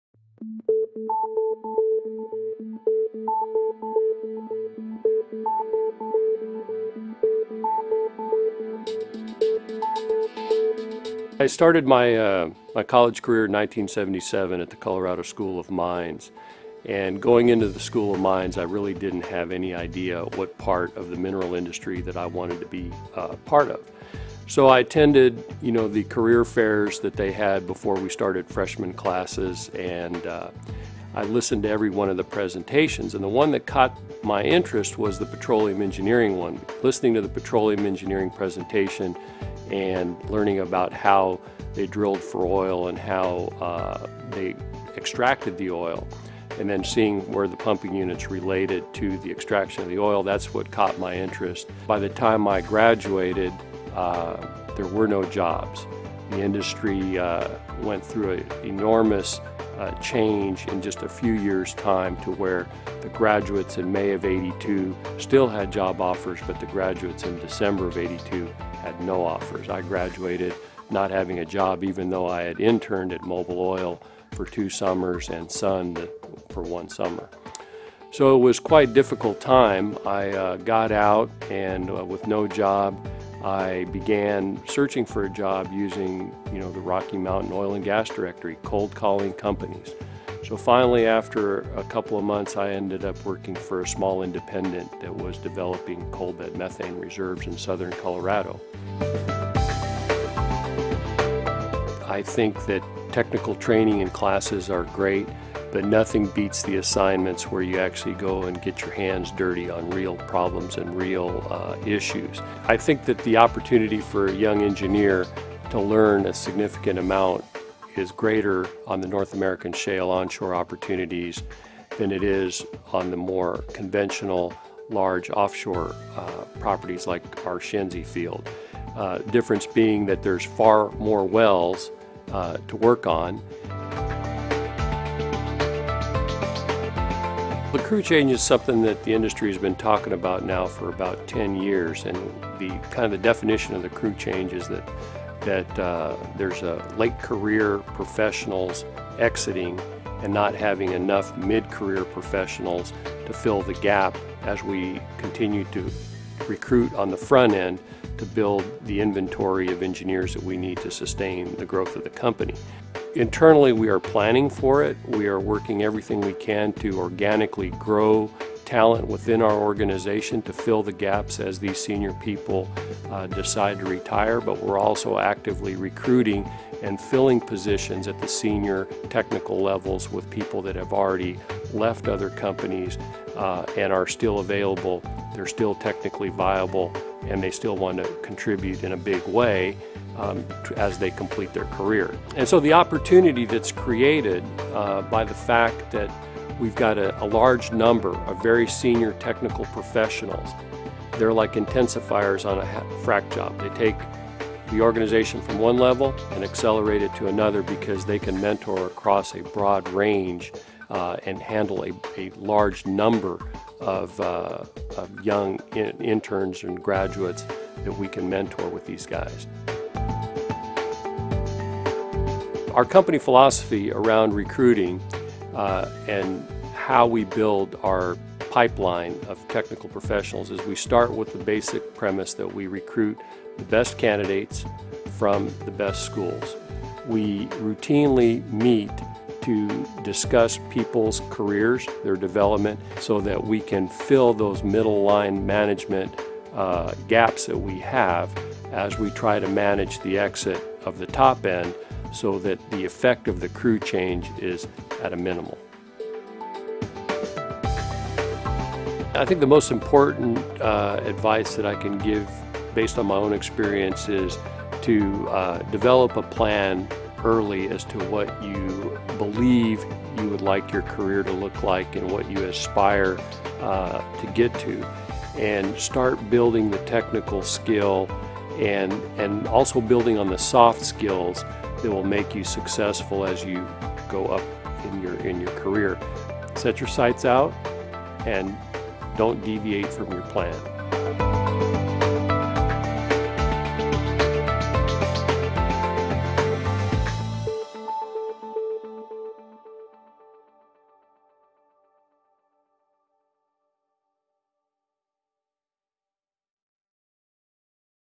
Industry Leaders: Your Start in the Oil and Gas Industry - Part Two
Industry Leader Robert Manelis shares his advice for getting started in the oil and gas industry.

Bob Manelis is vice president, central engineering, for BHP Billiton Petroleum in Houston, Texas. Bob has a BS in petroleum engineering from Colorado School of Mines.

With more than 30 years in the industry, he began his career with Mobil Oil in 1982, and gained increasing areas of responsibility in production engineering and reservoir engineering with ExxonMobil.

He has worked for BHP Billiton since 2005 in a variety of development planning, subsurface engineering, and pre-development engineering roles.

As vice president of central engineering, Bob has global responsibility for surface engineering and operations, production engineering, reservoir engineering, and petroleum reserves.